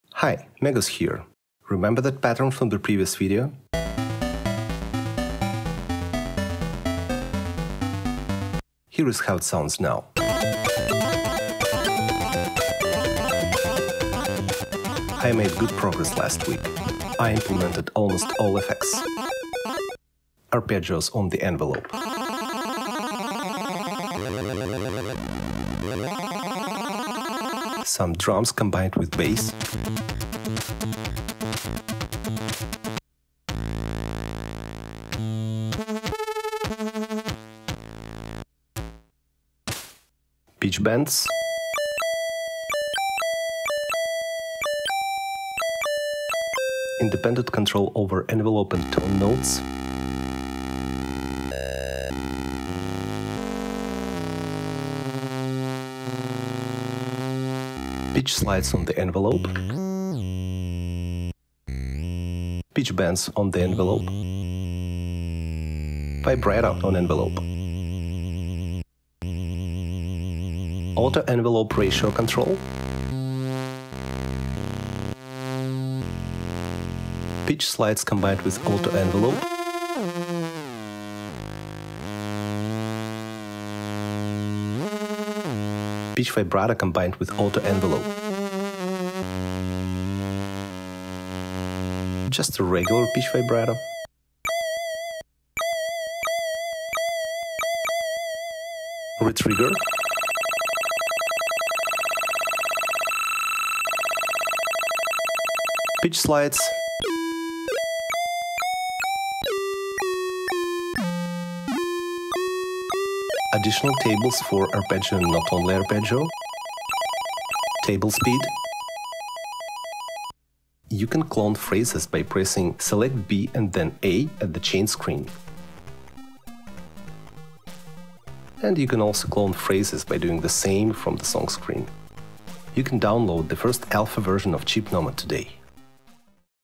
ChipNomad devlog and first alpha
ChipNomad is a multi-platform chiptune tracker. It is primarily targeted to handheld consoles like Anbernic RG35xx and uses the same UI paradigm as LSDJ and M8 Tracker.

The first supported chip is AY-3-8910/YM2149F because I come from the ZX Spectrum scene.

Today I'm releasing the first alpha version of ChipNomad. It is currently available for Anbernic RG35xx (and might also run on similar Linux-based handhelds like Miyoo Mini), Windows, and macOS (Apple Silicon).

I would love to hear your feedback!